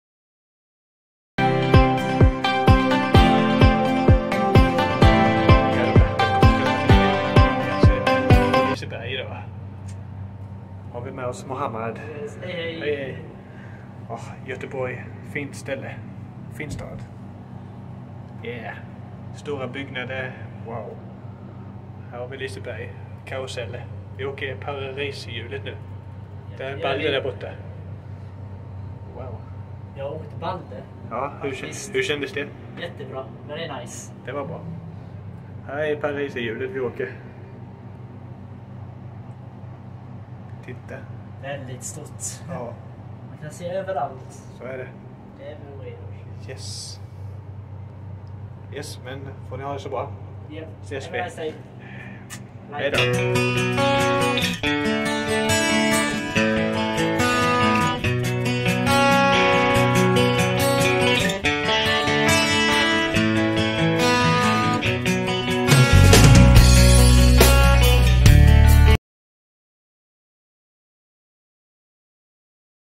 En liten video från Liseberg när vi åker pariserhjulet 🎡🎡🎡🎡👍👍👍👍
Det var riktigt roligt på Liseberg igår faktiskt 🎡🎡🎢🎢🎢🎡👍👍👍👍